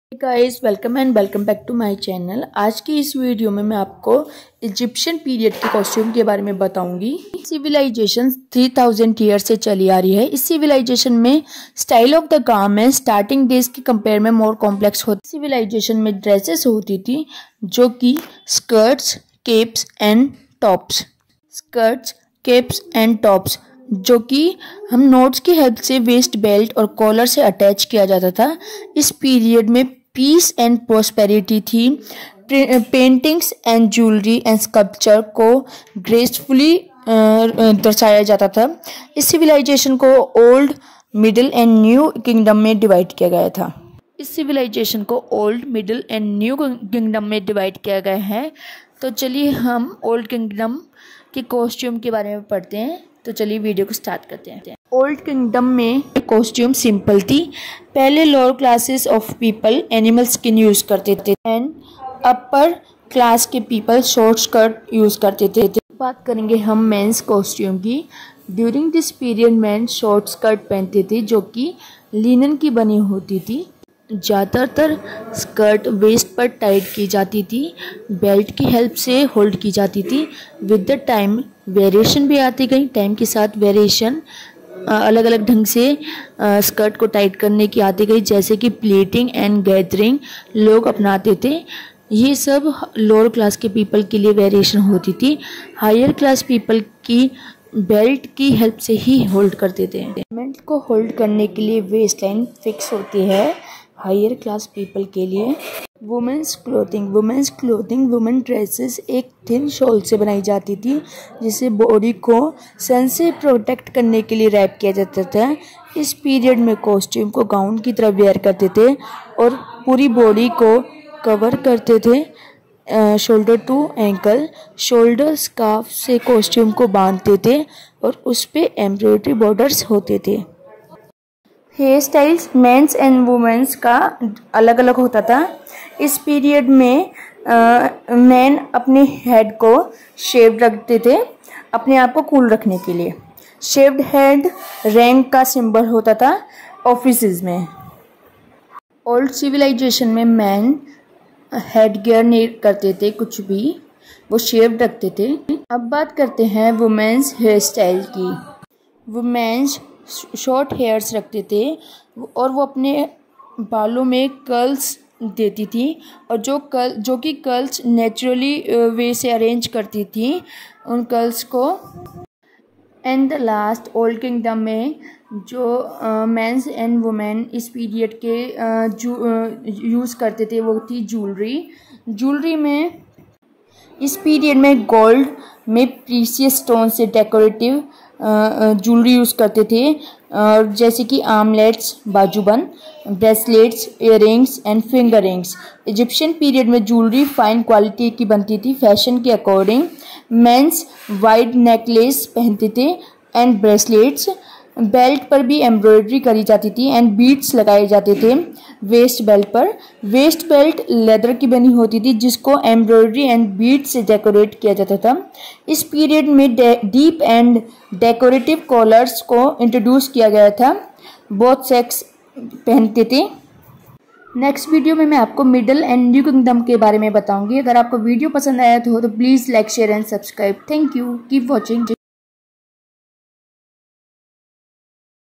Clothing in Ancient Egypt Or Costumes of Egyptian Civilization.मिस्र की सभ्यता के पोशाक विवरण
hi guys

is video me aapko Egyptian Civilization ke old kingdom ke costume le baare me jaan ne ko milega.

aapko esi trah ki videos aur chaiye ho to please comment box me btana.